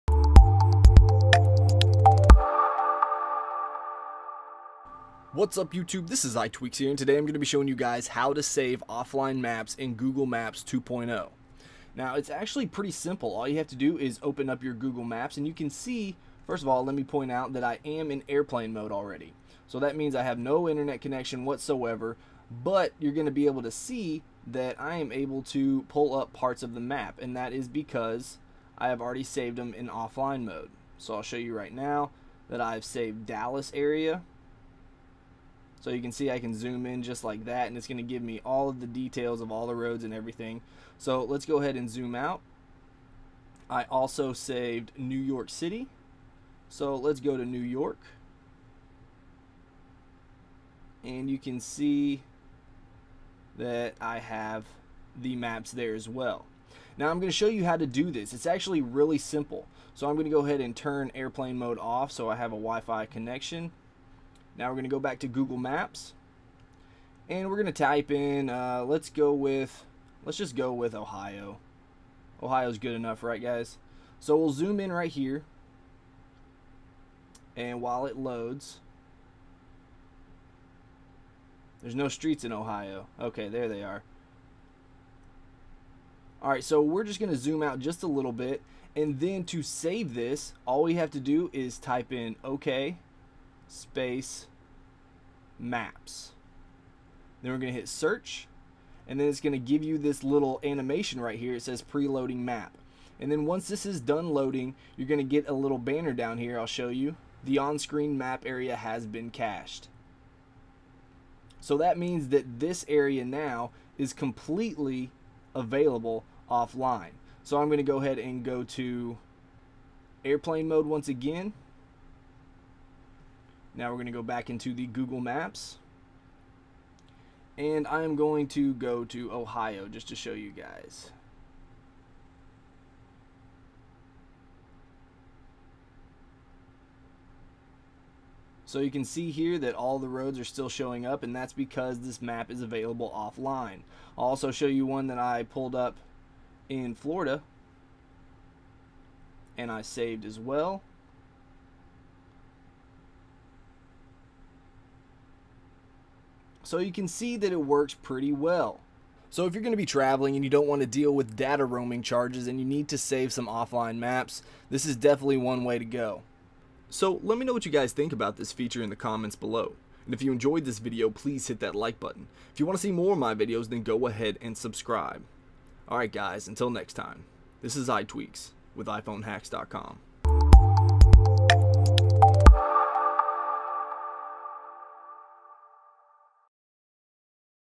How to save offline maps in Google Maps 2.0 for iPhone and iPad - iPhone Hacks
Google released Google Maps 2.0 which includes a number of new features and improvements including the "OK Maps" easter egg that allows you save offline maps.